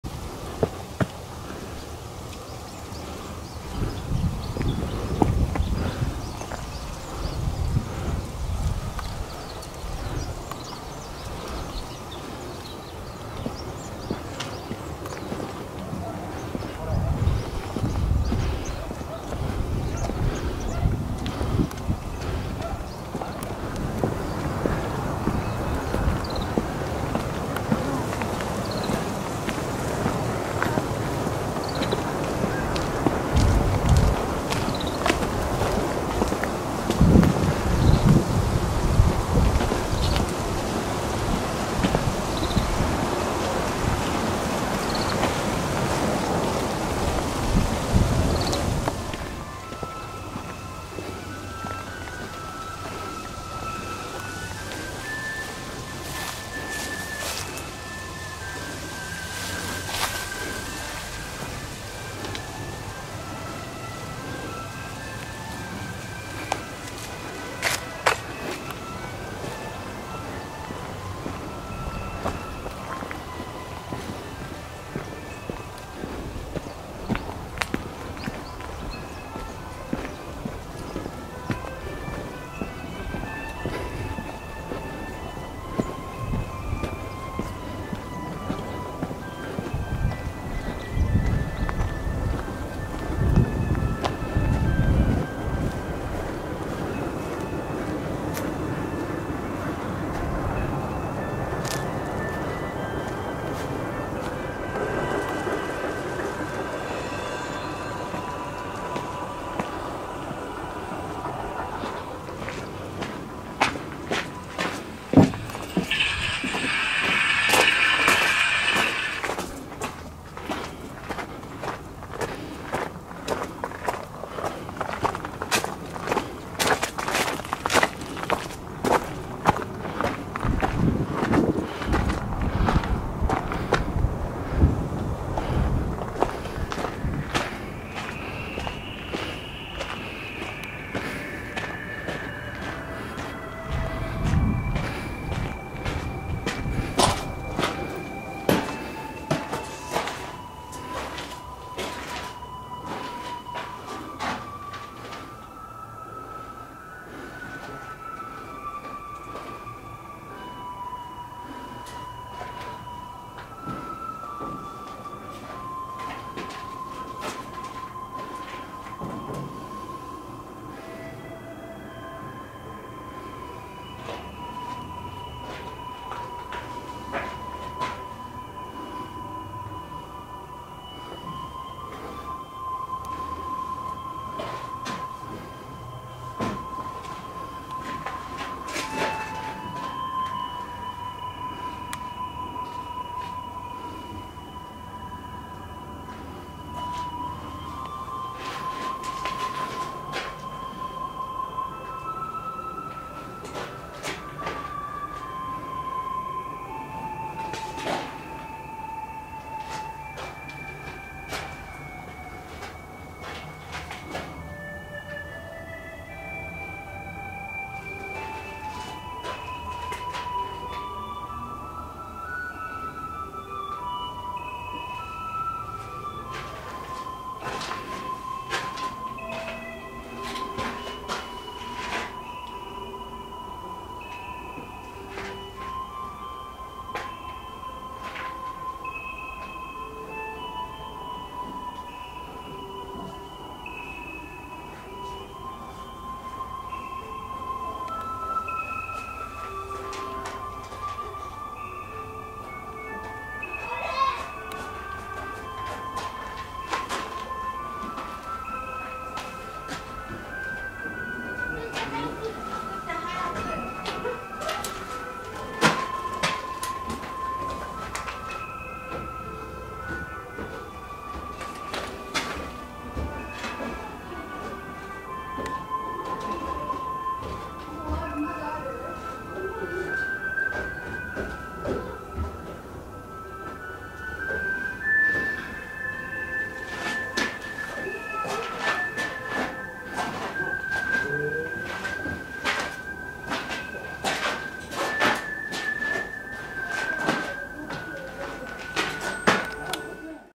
甦る五百年の歴史 荒砥城跡(Nagano) 千曲市城山史跡公園にて
荒砥城は、戦国時代に北信で最大の勢力を誇った村上氏の一族である山田氏の城で、村上氏は、武田の信濃侵攻に対抗し、居城であった葛尾城とは別にこの城を1524年に築城しました。
NHK大河ドラマ『風林火山」の撮影ロケ地として使用されました。